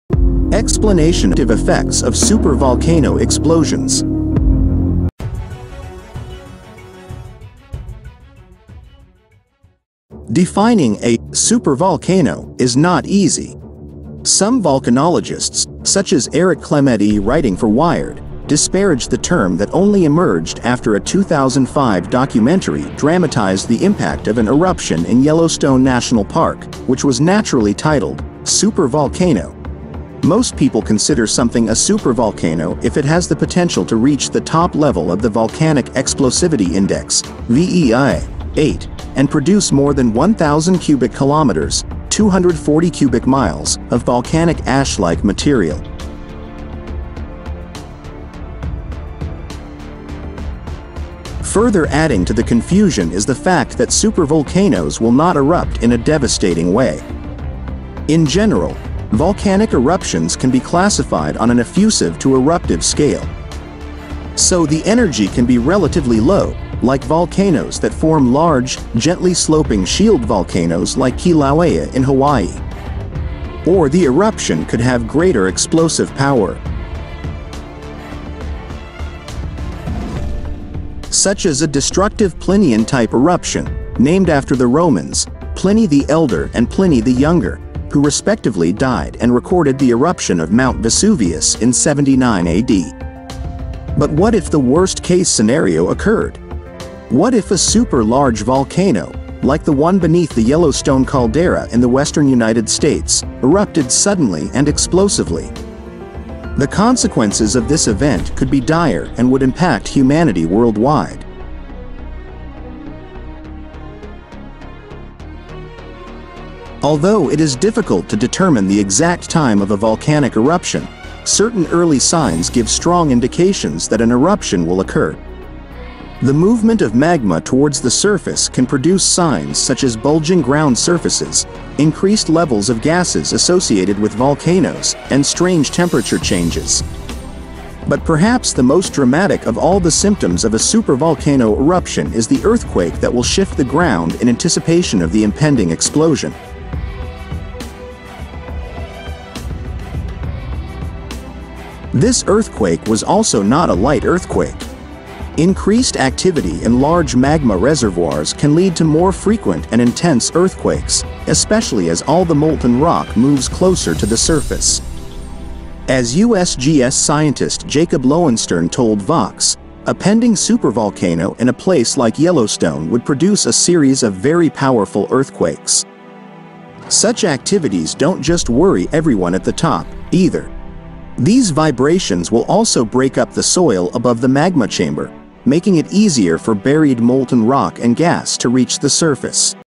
Scary today: The eruption of the Yellowstone Super Volcano had a bad impact on local residents
Defining a "supervolcano" is not easy. Some volcanologists, such as Erik Klemetti writing for Wired, disparage the term that only emerged after a 2005 documentary dramatized the impact of an eruption in Yellowstone National Park, which was naturally titled "Super Volcano." Most people consider something a supervolcano if it has the potential to reach the top level of the Volcanic Explosivity Index (VEI) (eight) and produce more than 1,000 cubic kilometers (240 cubic miles) of volcanic ash-like material.

Further adding to the confusion is the fact that supervolcanoes will not erupt in a devastating way. In general, volcanic eruptions can be classified on an effusive to eruptive scale. So the energy can be relatively low, like volcanoes that form large, gently sloping shield volcanoes like Kīlauea in Hawaii. Or the eruption could have greater explosive power.

Such as a destructive Plinian-type eruption (named after the Romans, Pliny the Elder and Pliny the Younger, who respectively died and recorded the eruption of Mount Vesuvius in 79 AD). But what if the worst-case scenario occurred? What if a super-large volcano, like the one beneath the Yellowstone caldera in the western United States, erupted suddenly and explosively? The consequences of this event could be dire and would impact humanity worldwide.